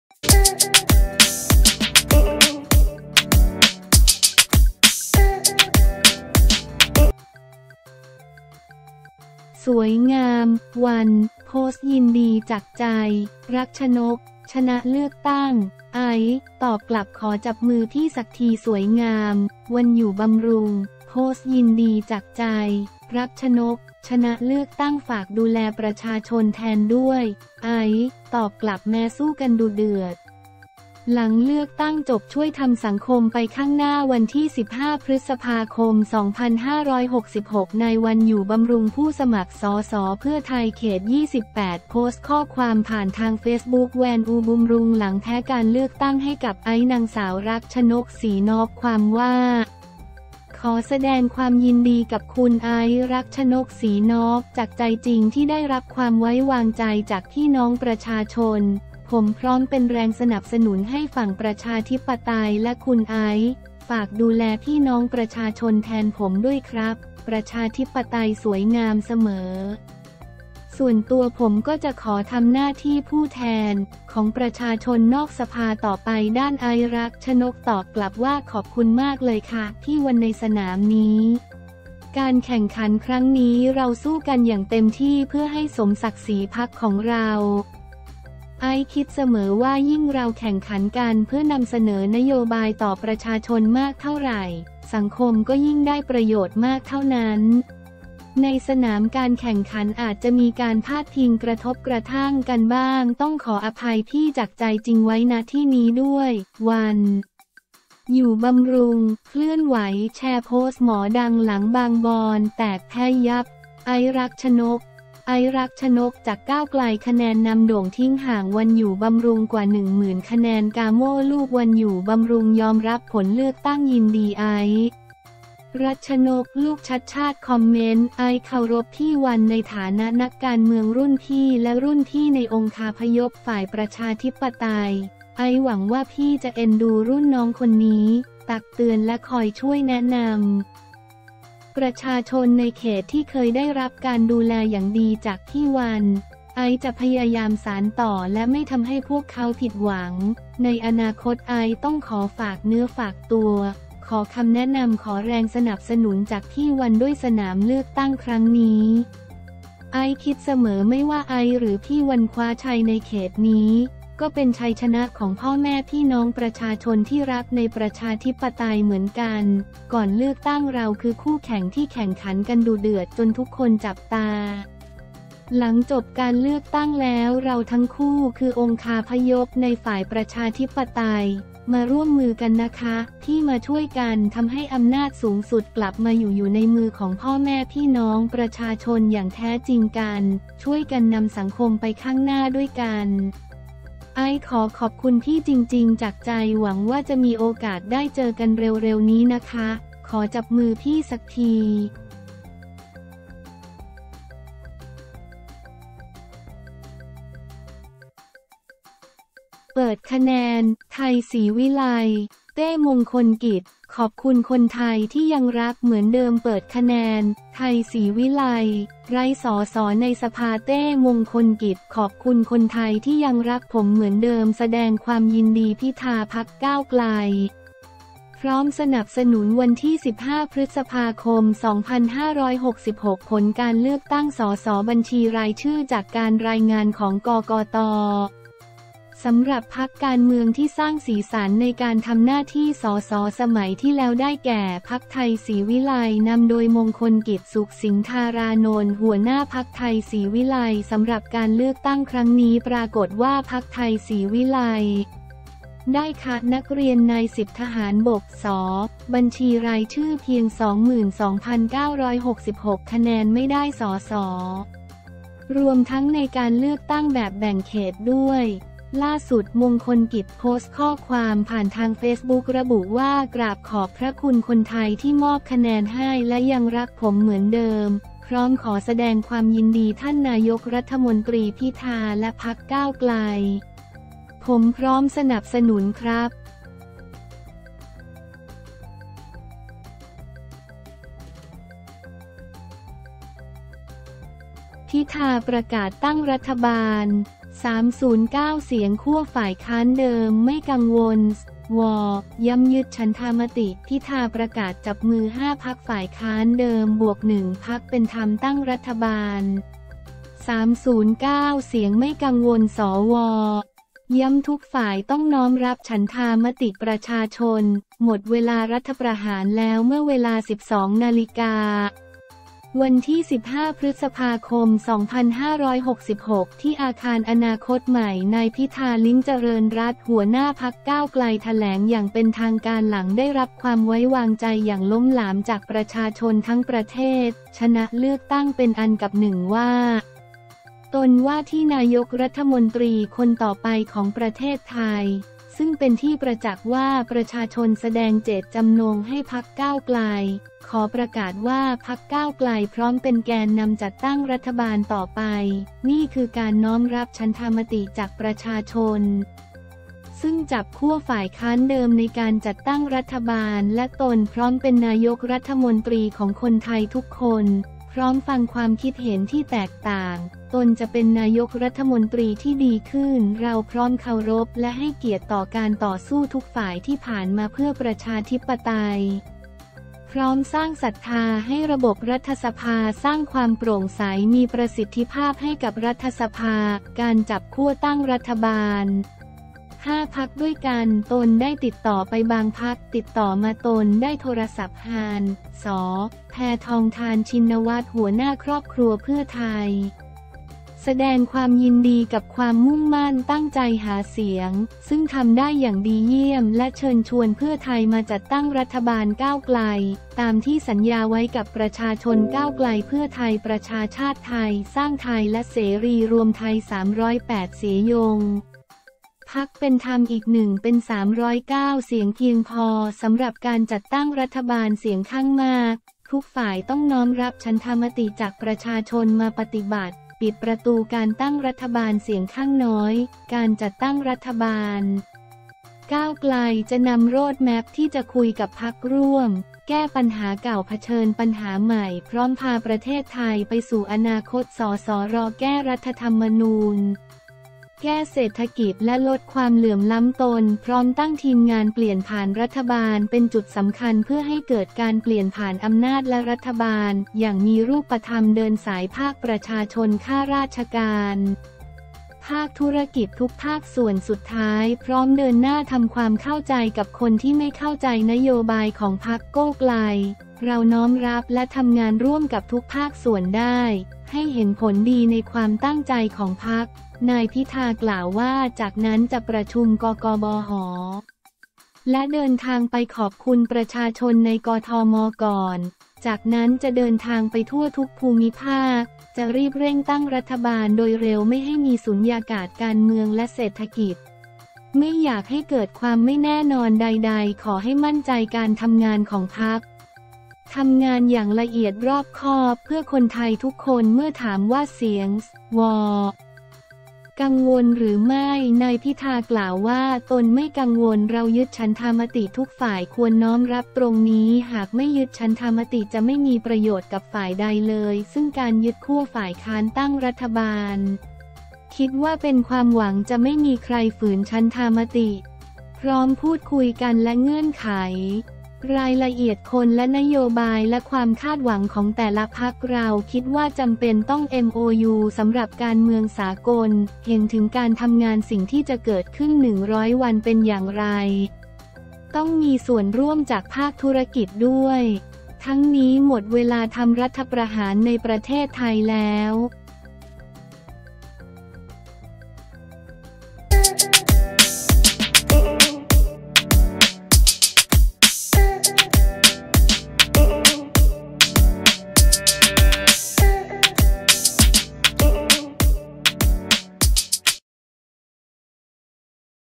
สวยงาม 'วัน' โพสต์ยินดีจากใจ 'รักชนก' ชนะเลือกตั้ง 'ไอซ์' ตอบกลับ ขอจับมือพี่สักที
สวยงาม ‘วัน อยู่บำรุง’ โพสต์ยินดีจากใจ ‘รักชนก’ ชนะเลือกตั้ง ฝากดูแลประชาชนแทนด้วย ‘ไอซ์’ ตอบกลับ แม้สู้กันดุเดือด หลังเลือกตั้งจบ ช่วยทำสังคมไปข้างหน้า
วันที่ 15 พ.ค.2566 นายวัน อยู่บำรุง ผู้สมัคร ส.ส.เพื่อไทย เขต 28 โพสต์ข้อความผ่านทางเฟซบุ๊ก Wan Ubumrung หลังแพ้การเลือกตั้งให้กับ ไอซ์ น.ส.รักชนก ศรีนอก ความว่า ขอแสดงความยินดีกับคุณไอซ์ รักชนก ศรีนอก จากใจจริงที่ได้รับความไว้วางใจจากพี่น้องประชาชน ผมพร้อมเป็นแรงสนับสนุนให้ฝั่งประชาธิปไตยและคุณไอซ์ ฝากดูแลพี่น้องประชาชนแทนผมด้วยครับ ประชาธิปไตยสวยงามเสมอ ส่วนตัวผมก็จะขอทำหน้าที่ผู้เแทน ของประชาชนนอกสภาต่อไป
ด้าน ไอซ์ รักชนก ตอบกลับว่า ขอบคุณมากเลยค่ะพี่วัน ในสนามนี้ การแข่งขันครั้งนี้เราสู้กันอย่างเต็มที่เพื่อให้สมศักดิ์ศรีพรรคของเรา ไอซ์คิดเสมอว่ายิ่งเราแข่งขันกันเพื่อนำเสนอนโยบายต่อประชาชนมากเท่าไหร่ สังคมก็ยิ่งได้ประโยชน์มากเท่านั้น ในสนามการแข่งขันอาจจะมีการพาดพิงกระทบกระทั่งกันบ้างต้องขออภัยพี่จากใจจริงไว้ ณ ที่นี้ด้วย

‘วัน อยู่บำรุง’ เคลื่อนไหว แชร์โพสต์หมอดัง หลังบางบอนแตก แพ้ยับ ‘ไอซ์ รักชนก’
ไอซ์ รักชนก จากก้าวไกล คะแนนนำโด่ง ทิ้งห่าง วัน อยู่บำรุง กว่า 1 หมื่นคะแนน
กาโม ลูกวัน อยู่บำรุง ยอมรับผลเลือกตั้ง ยินดีไอซ์ รัชนก ลูกชัชชาติคอมเมนท์
ไอซ์เคารพพี่วันในฐานะนักการเมืองรุ่นพี่และรุ่นพี่ในองคาพยพฝ่ายประชาธิปไตย ไอซ์หวังว่าพี่จะเอ็นดูรุ่นน้องคนนี้ ตักเตือนและคอยช่วยแนะนำ ประชาชนในเขตที่เคยได้รับการดูแลอย่างดีจากพี่วัน ไอซ์จะพยายามสานต่อและไม่ทำให้พวกเค้าผิดหวัง ในอนาคตไอซ์ต้องขอฝากเนื้อฝากตัว ขอคำแนะนำ ขอแรงสนับสนุนจากพี่วันด้วย
สนามเลือกตั้งครั้งนี้ ไอซ์คิดเสมอไม่ว่าไอซ์หรือพี่วันคว้าชัยในเขตนี้ ก็เป็นชัยชนะของพ่อแม่พี่น้องประชาชนที่รักในประชาธิปไตยเหมือนกัน ก่อนเลือกตั้งเราคือคู่แข่ง ที่แข่งขันกันดุเดือดจนทุกคนจับตา หลังจบการเลือกตั้งแล้วเราทั้งคู่คือองคาพยพในฝ่ายประชาธิปไตย มาร่วมมือกันนะคะพี่ มาช่วยกันทำให้อำนาจสูงสุดกลับมาอยู่อยู่ในมือของพ่อแม่พี่น้องประชาชนอย่างแท้จริงกัน ช่วยกันนำสังคมไปข้างหน้าด้วยกัน

ไอซ์ขอขอบคุณพี่จริงๆจากใจ หวังว่าจะมีโอกาสได้เจอกันเร็วๆนี้นะคะ ขอจับมือพี่สักที